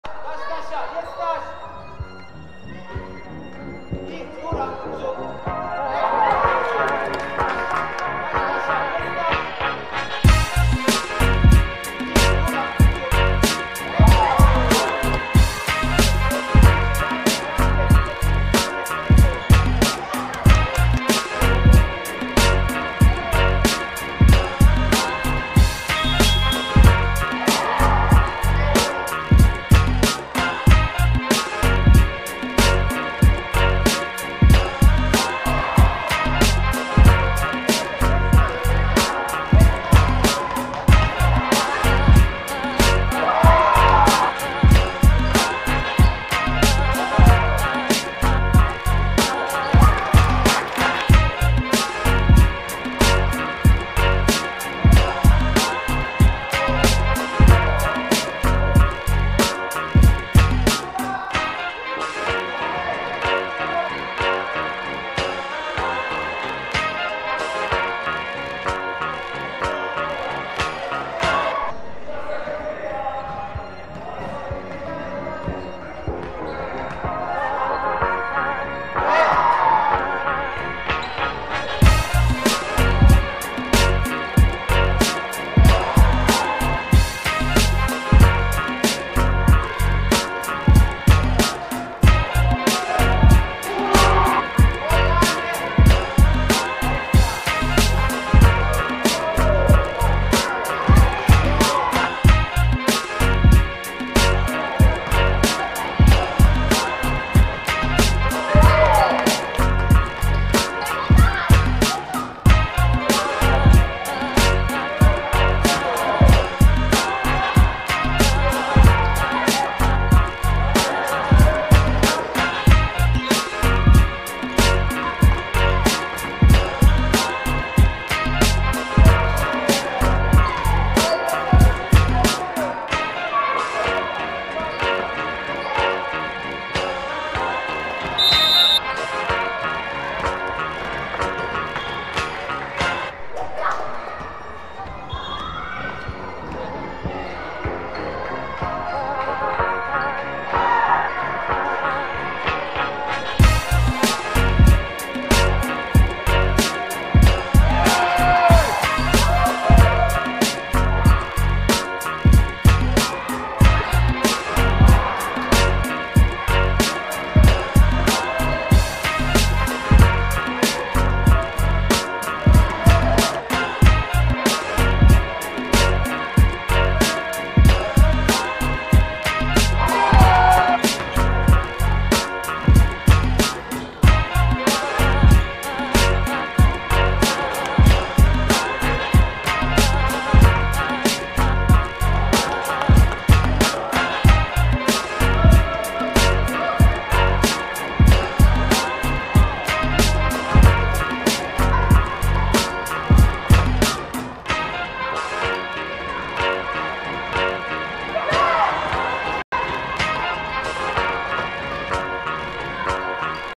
10 10 Kobry vs Kangury – Dzika Liga Junior 15
Kobry 21:47 Kangury
W.Rudawski 10pkt 4ast 10stl
S.Ciesielski 10pkt 5zb 5stl